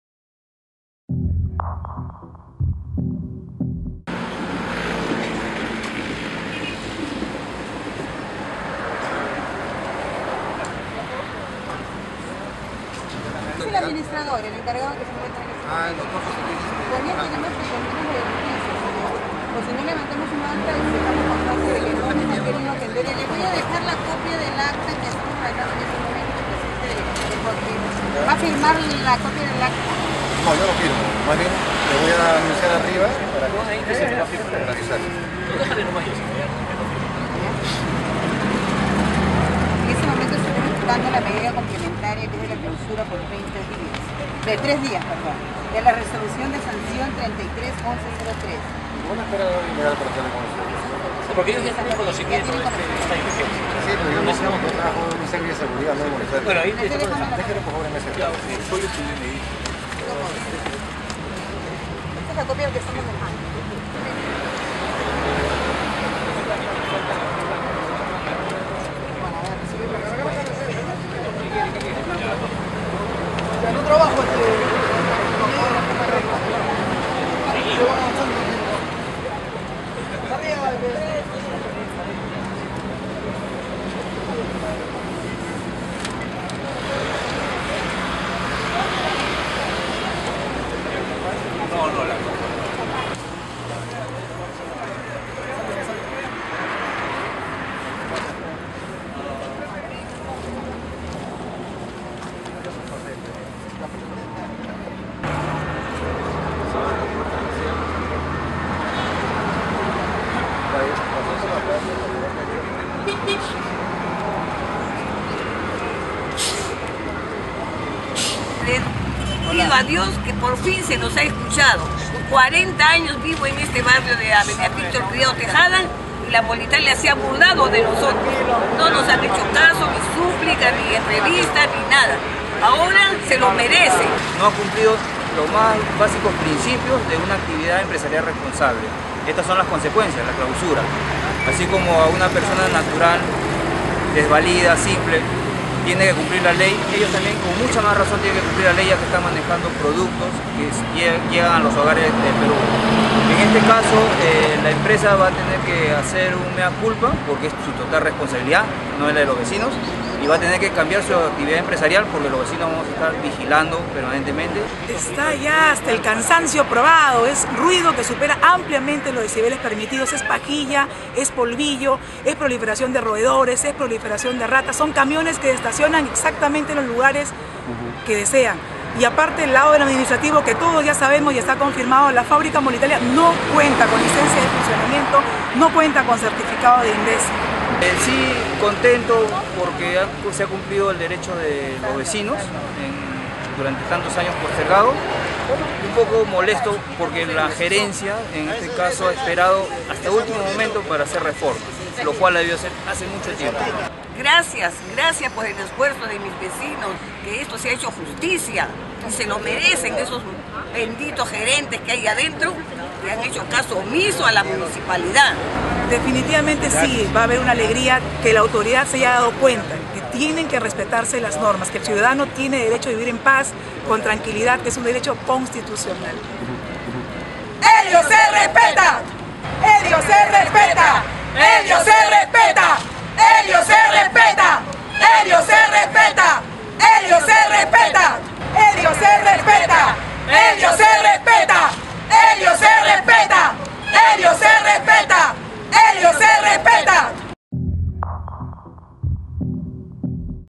Municipalidad de Lima cierra Molitalia por contaminación
día de hoy, aproximadamente a las 4:20 pm, la Gerencia de Fiscalización de la Municipalidad de Lima finalmente clausuró la fábrica de la empresa Molitalia, ubicada en la urbanización Elio (Cercado de Lima) debido a que se comprobó que el complejo generaba contaminación sonora, superando los estándares de calidad ambiental de ruido permitidos, así como de los niveles recomendados por la Organización Mundial de la Salud. Asimismo, le impuso una multa de 1,850 nuevos soles.